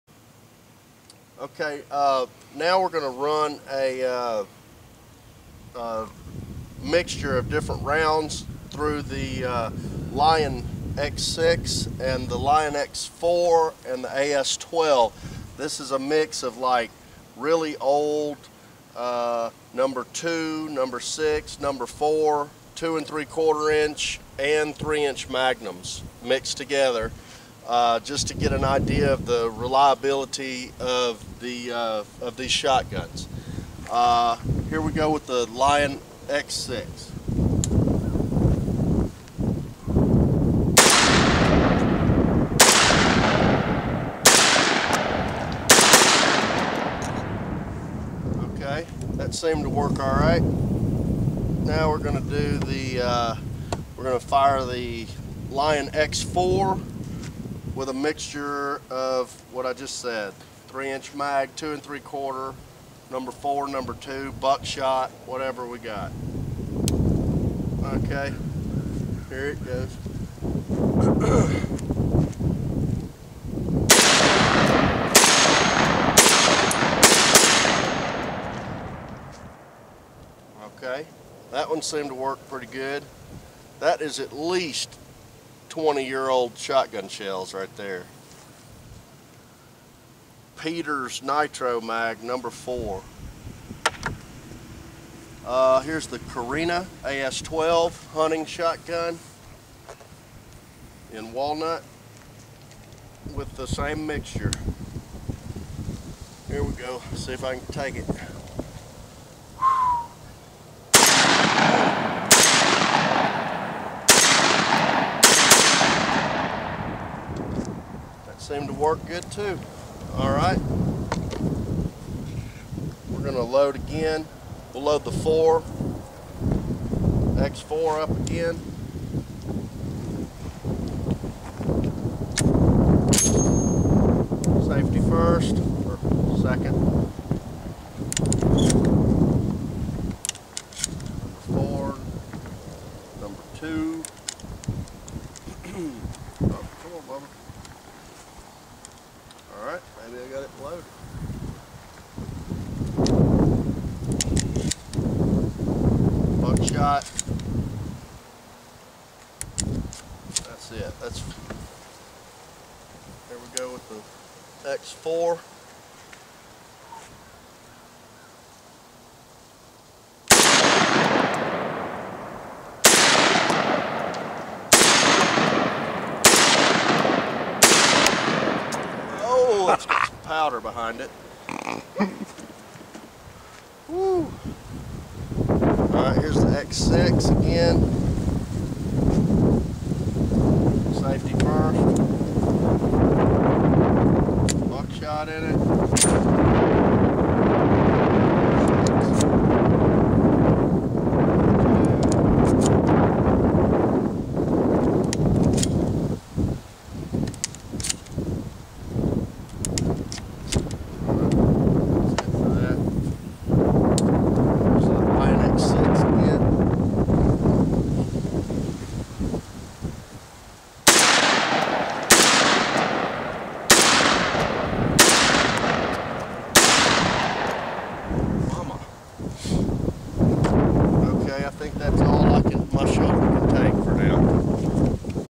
Tactical & Hunting shotgun Testing Lion x6, x4,& carina AS 12 Advanced Tactical Imports
Reliability testing with mixed shot-shells. Lion x6, Lion x4 and Carina AS-12 by Derya Arms. Ammunition is 15-25 year old #2, #4, #6, #00 buck, 3 inch and 2 3/4 inch, medium to heavy loads.